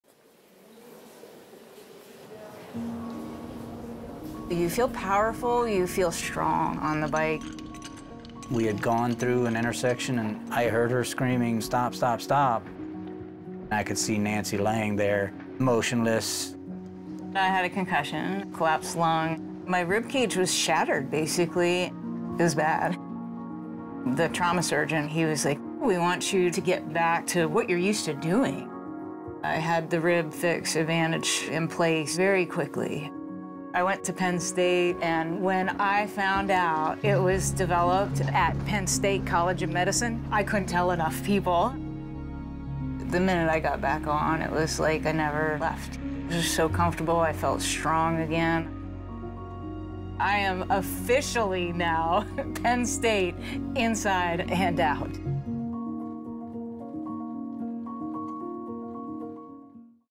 Breakthrough Medicine Saves Team USA Triathlete Nancy Rehm's Life After SUV Accident
In 2024, Team USA triathlete Nancy Rehm suffered more than 20 broken bones after being struck by an SUV. Her life was saved by a breakthrough approach developed at Penn State College of Medicine: RibFix Advantage™.

This story shows how academic research becomes real-world care, helping Nancy recover and thrive, not just survive. Within months of her surgery, she was back to competing and represented Team USA at the Age-Group Multisport World Championships.

Learn more about how breakthrough medicine is powered by Penn State at BreakthroughMedicine.org